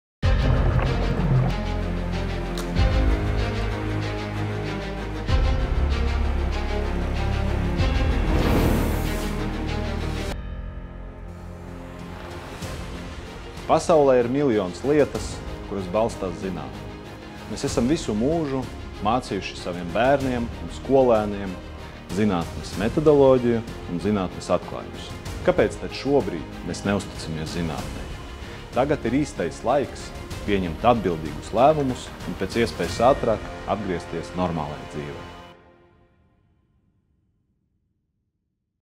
Pāvels Pestovs: "Uzticies zinātnei un atgriezies normālā dzīvē!"
Covid-19 ir vīrusa infekcijas slimība ar ļoti augstu lipīguma pakāpi, kura pirms pusotra gada pasaulē izraisīja pandēmiju, kas joprojām ietekmē visas pasaules sabiedrības ierasto dzīves ritmu, ikviena cilvēka personīgo dzīvi un sabiedriskās aktivitātes.

Vīruss ir dzīvs organisms un darbojas kā parazīts, kuram, lai dzīvotu un turpinātu savu dzīves ciklu, ir nepieciešams saimnieka organisms - šīs infekcijas gadījumā tas ir cilvēks. Vakcinēts cilvēks, kuram pret šo vīrusu izveidojušās antivielas, nomāc vīrusa veidošanos cilvēka organismā vai pilnībā vīrusu iznīcina. Vīruss iet bojā vai novājinās, un līdz ar to tiek pārtraukta vīrusu pārvietošanās ķēde no viena cilvēka uz otru un vīrusu cirkulācija sabiedrībā.

Pasaulē ir bijušas daudzas pandēmijas, un tikai vakcinācija ir likvidējusi tādas saslimšanas kā bakas, poliomielīti, samazinājusi saslimšanu ar daudzām bērnu infekcijas slimībām pasaulē. Aicinām izglītības iestāžu darbiniekus būt sociāli atbildīgiem un vakcinēties, lai atjaunotu iespēju sev un pārējiem kolēģiem organizēt kvalitatīvu izglītības procesu klātienē. Izmantojiet ātrāko iespēju, kāda šobrīd ir pieejama, lai pārtrauktu vīrusu pārvietošanās ķēdi, un vakcinējieties!

Zvaniet pa bezmaksas informatīvo vakcinācijas tālruni 8989, lai ar operatoru palīdzību izvēlētos konkrēto vakcinācijas vietu un laiku!